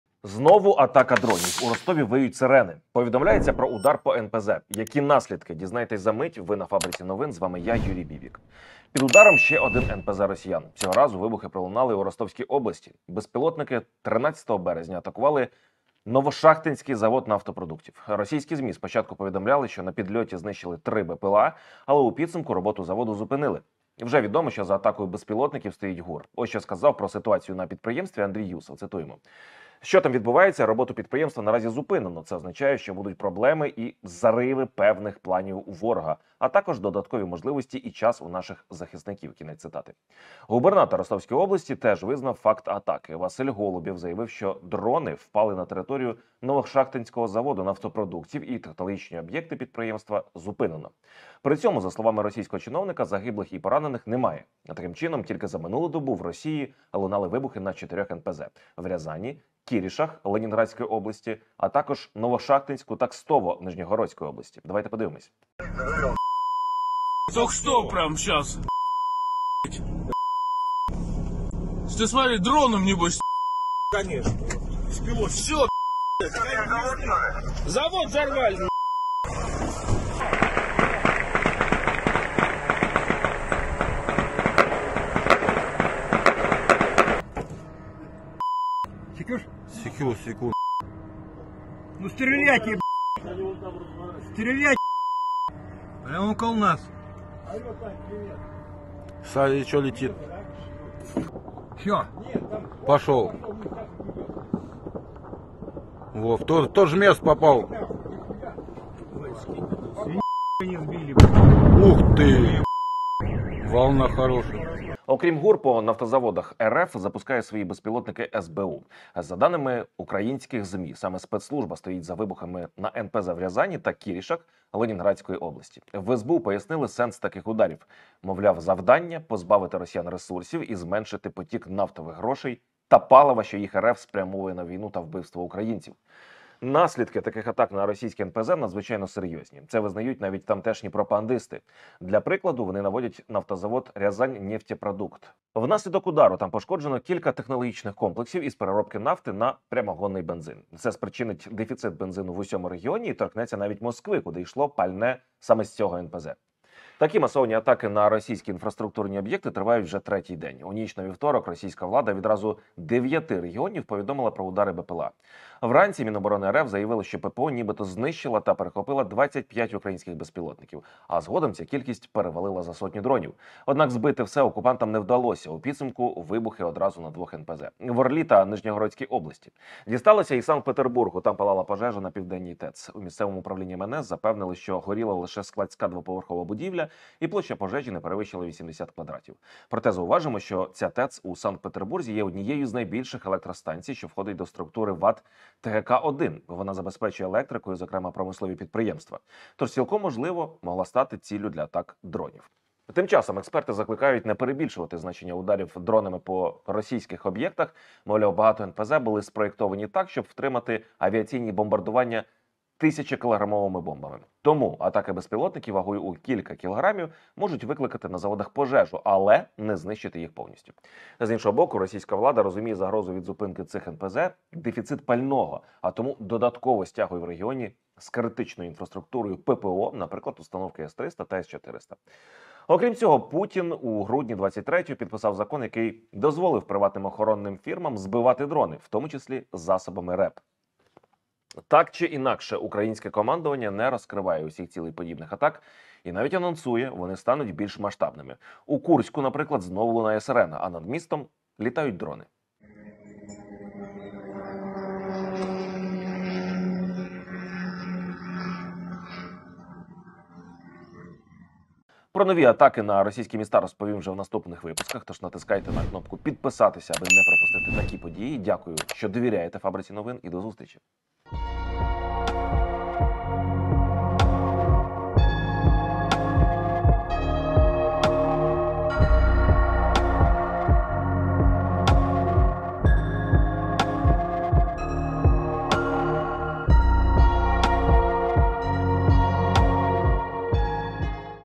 💥Москву накриє НОВИЙ КОЛАПС! Підірвали три заводи. В ГУР анонсували катастрофу в Росії
Під ударом ще один НЗП росіян! Цього разу вибухи пролунали у Ростовській області, атакували Новошахтинський завод нафтопродуктів. В ГУР заявили, що роботу підприємства наразі зупинено. СБУ також вдарило по РФ - горіли нафтові заводи в Рязані та Ленінградській області. У спецслужбі назвали головну мету цих атак. Спецслужби прогнозують дефіцит бензину у Москві та інших регіонах.